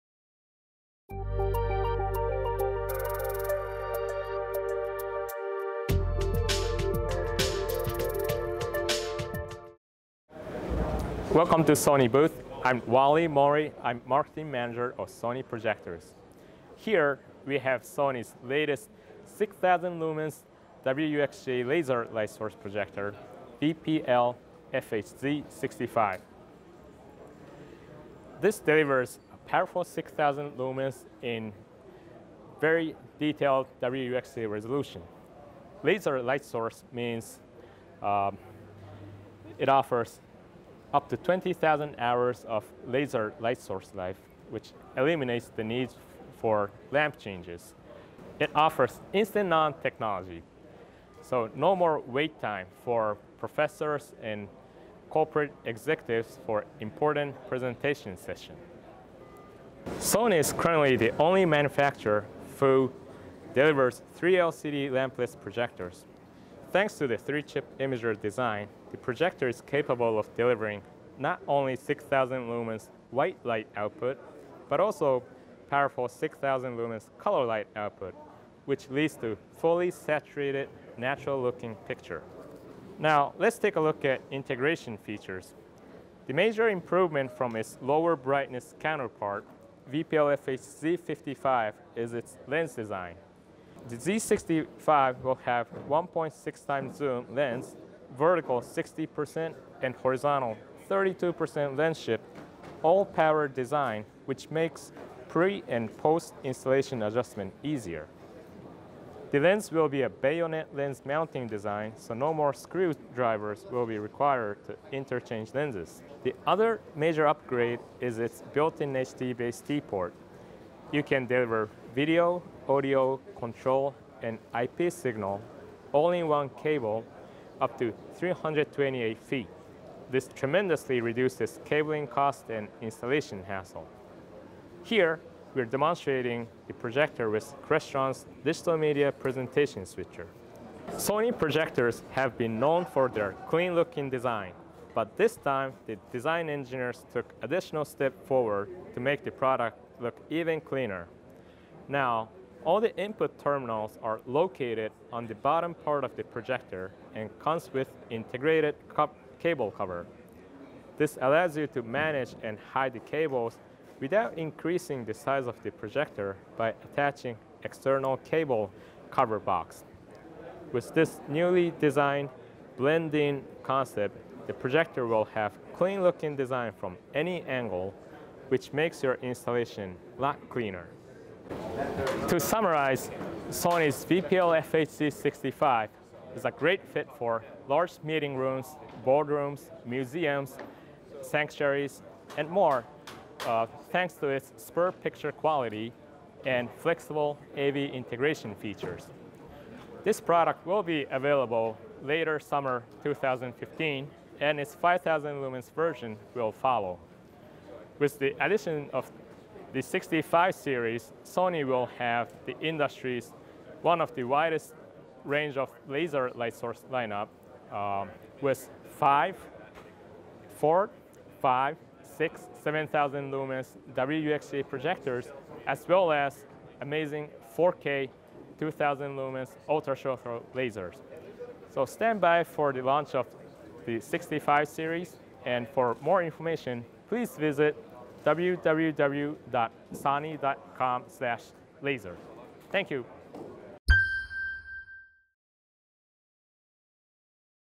VPL FHZ65 - 6000lm/WUXGA Laser Light Source Projector
Combines Sony's 3LCD BrightEra technology with Z-Phosphor Laser Light Source Engine